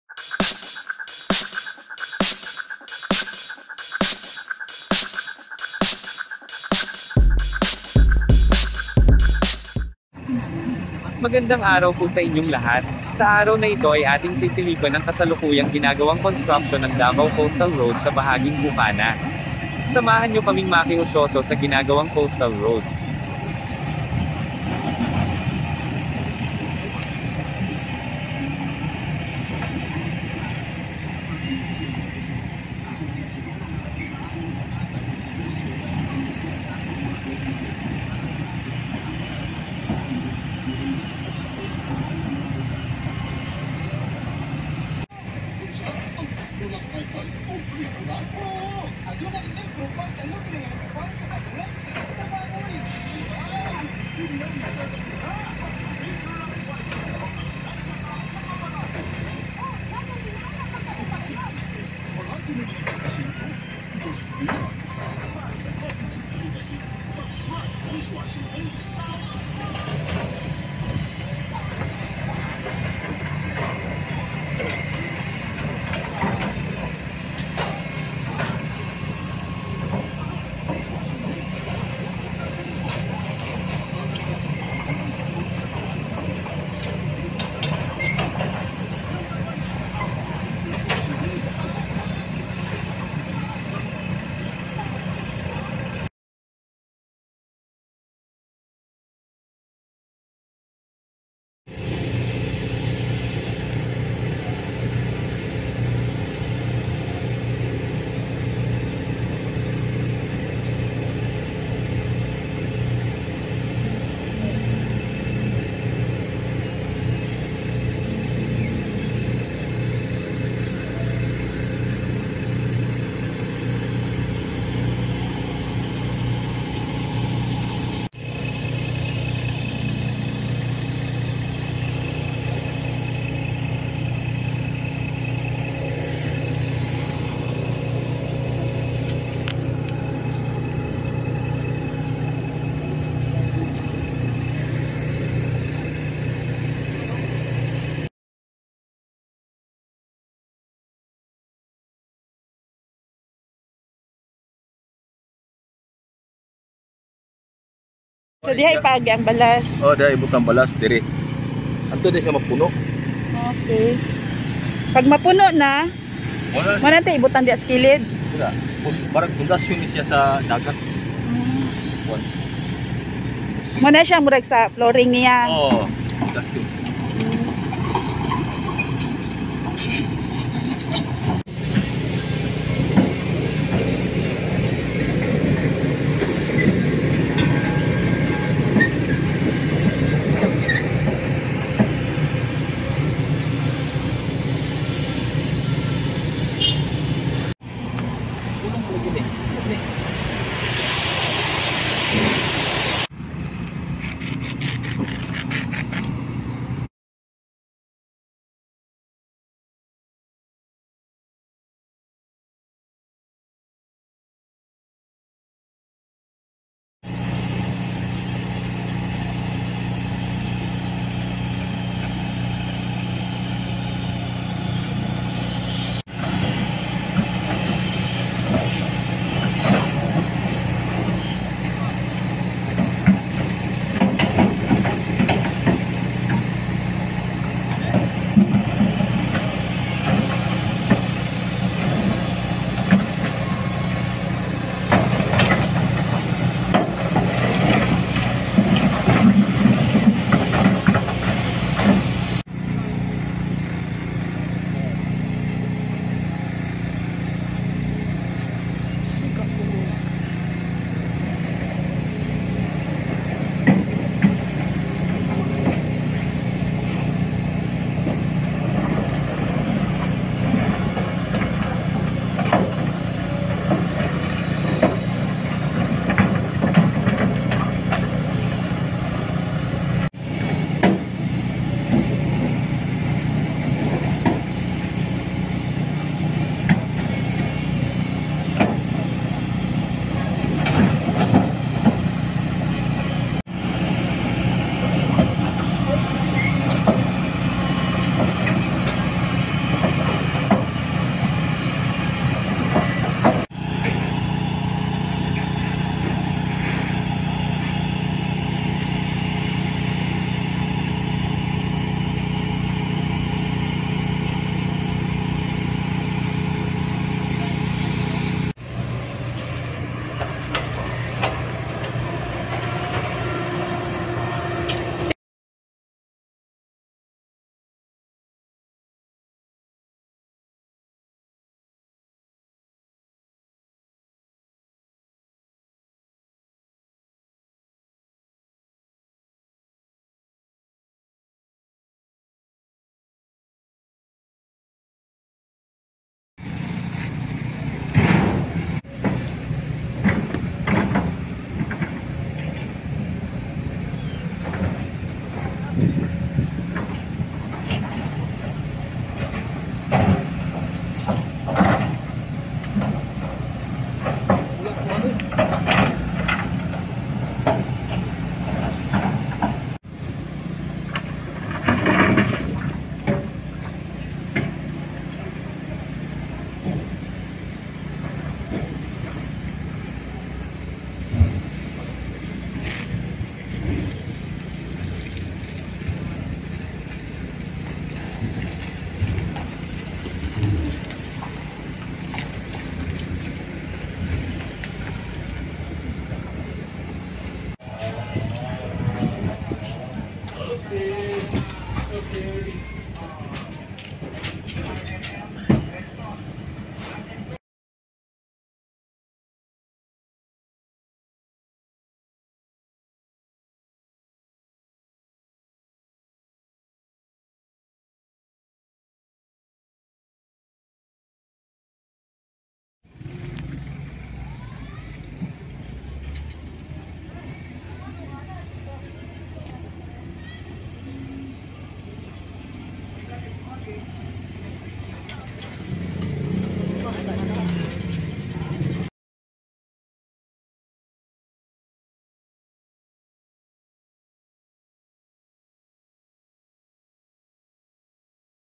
A Quick Peek on The Davao Coastal Road, Bucana Segment while under Construction
In this video, we will have a quick peek at the Davao Coastal Road while there is ongoing construction. We will watch the Construction in Action.